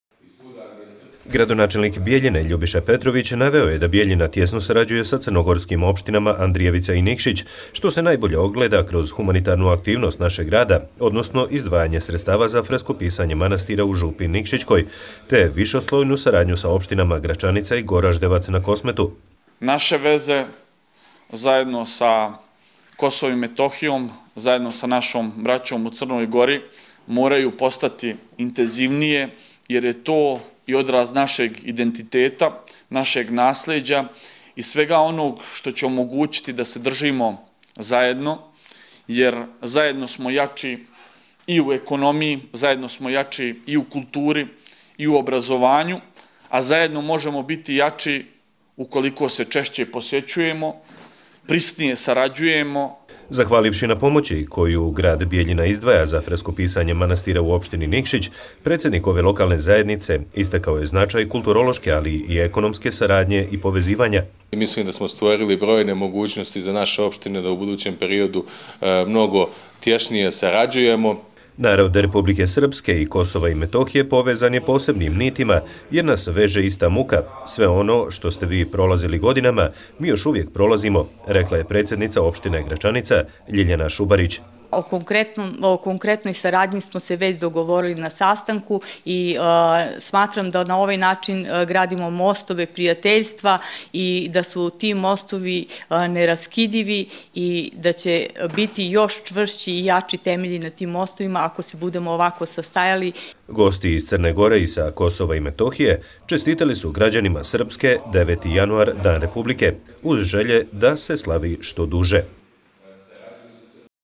Bijeljina – Prijem za predstavnike bratskih i prijateljskih gradova iz Crne Gore i sa Kosova
U bijeljinskoj gradskoj upravi juče je organizovan prijem za predstavnike bratskih i prijateljskih opština iz Crne Gore i južne srpske pokrajine – Kosova i Metohije, na kom je razgovarano o kulturnoj i ekonomskoj saradnji.